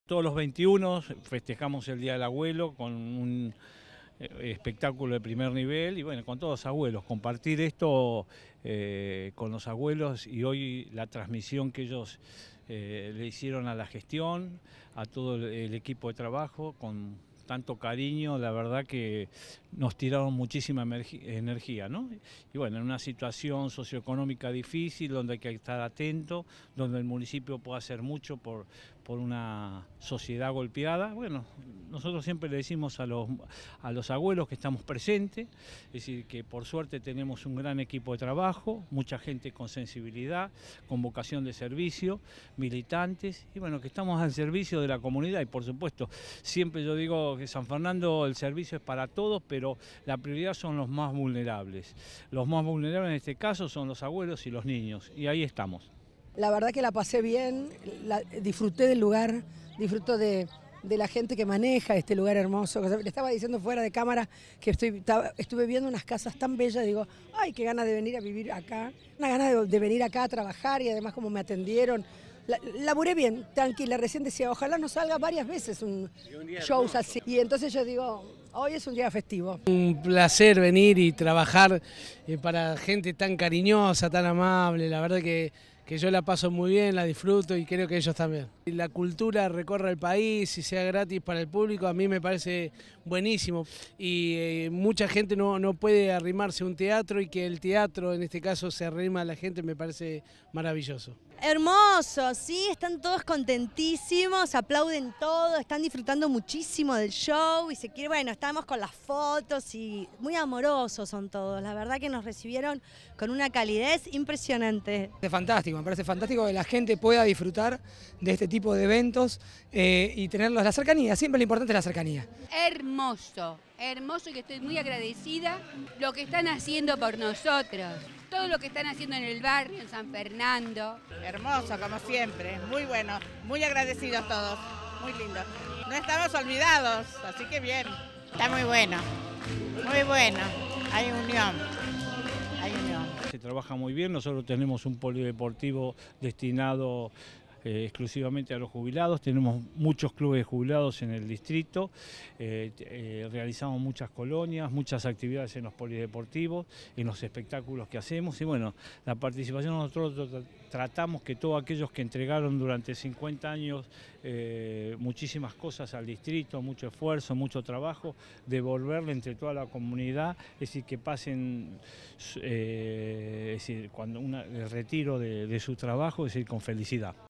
Andreotti agasajó a miles de abuelos sanfernandinos por el Día del Jubilado
El intendente de San Fernando celebró junto 3.000 adultos mayores de clubes y polideportivos un inolvidable encuentro en el Parque Náutico, con un gran almuerzo y un show sorpresa de la vedette Carmen Barbieri, el humorista Marcos ‘Bicho’ Gómez, y los bailarines Valeria Archimó y Juampi Battaglia.